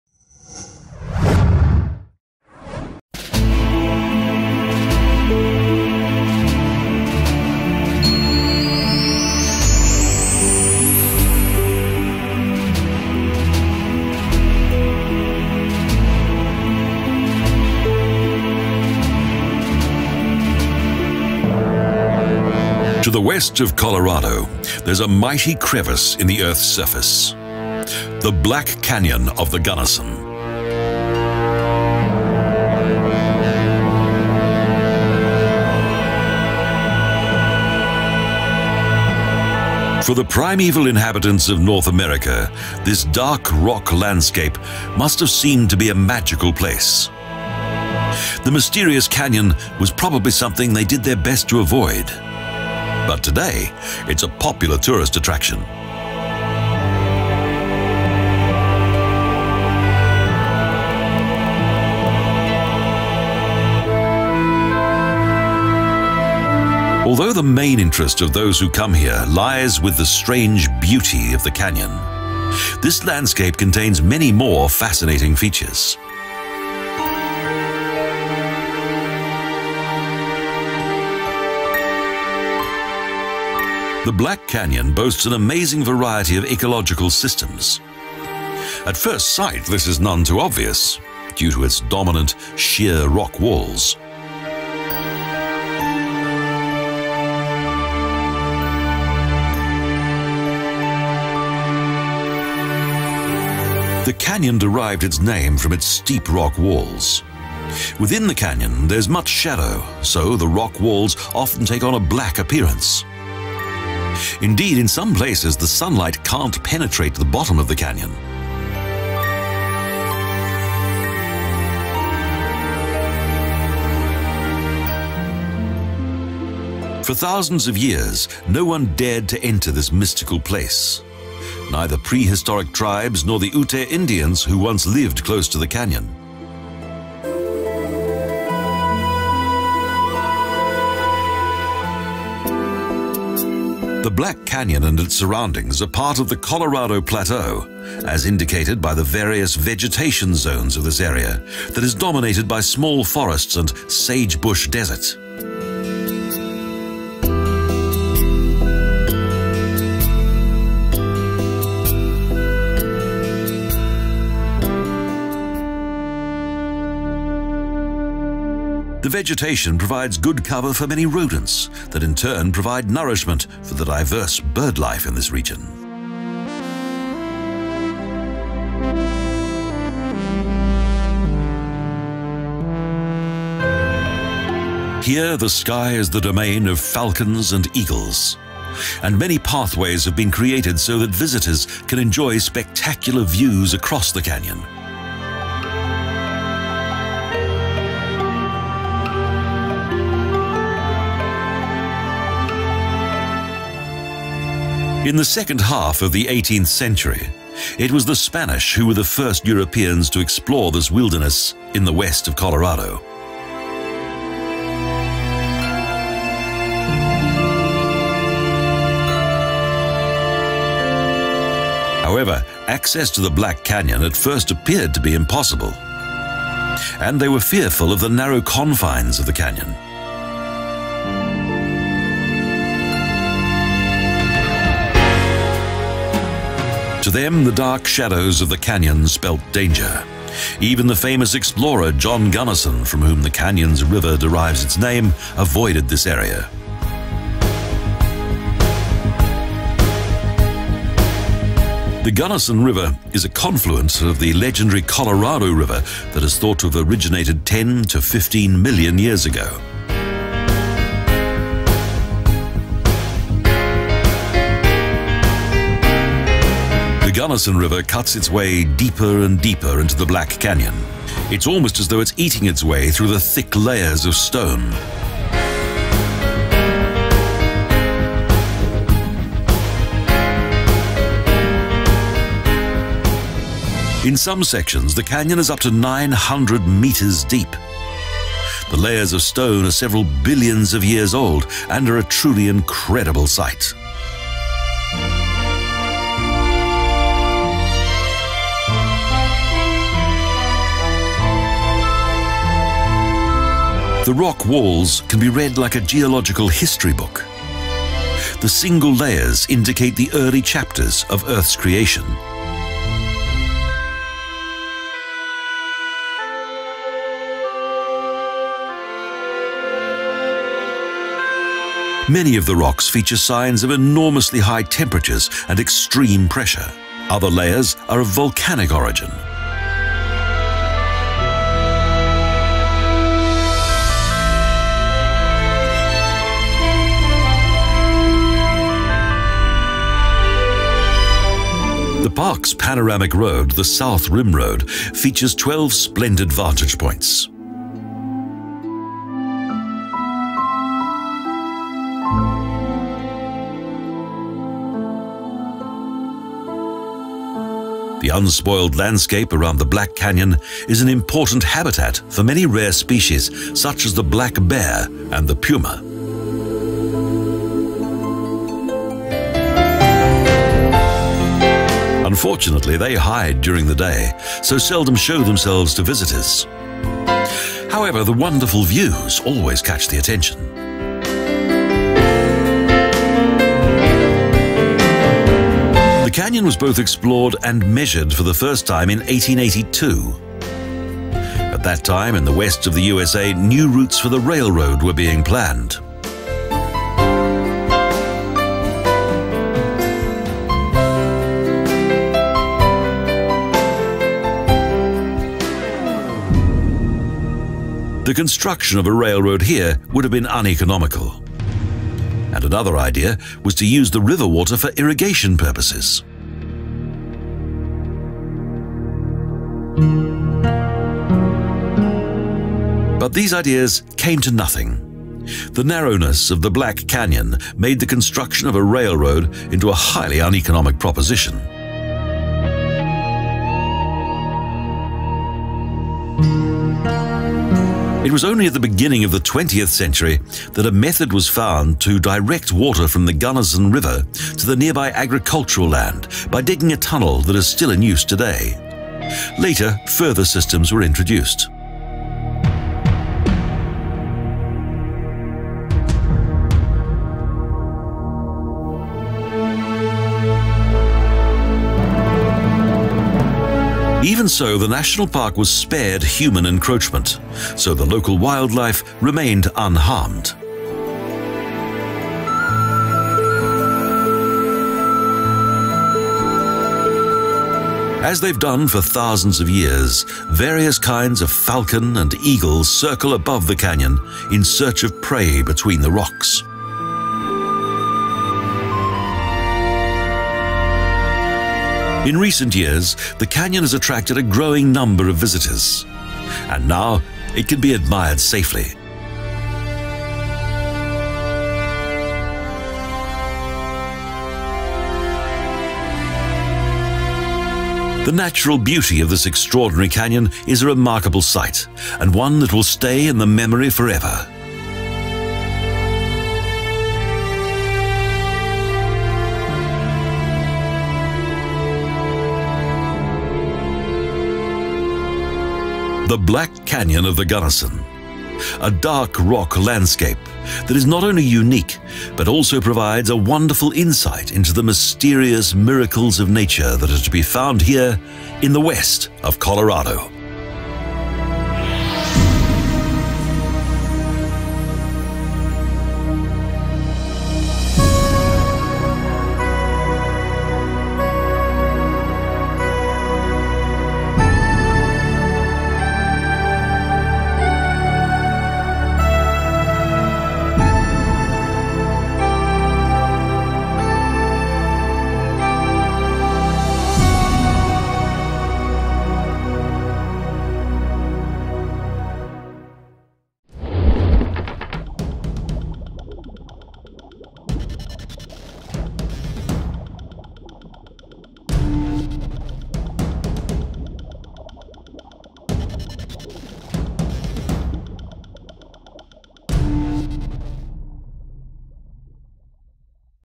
Black Canyon Of The Gunnison Vacation Travel Video Guide
Travel video about destination Black Canyon of the Gunnison.
In the west of Colorado there is a mighty crevice in the earth's surface, the Black Canyon Of The Gunnison. For the primeval inhabitants of North America this dark rock landscape must have seemed to be a magical place and was probably something they did their best to avoid but today it is a popular tourist attraction.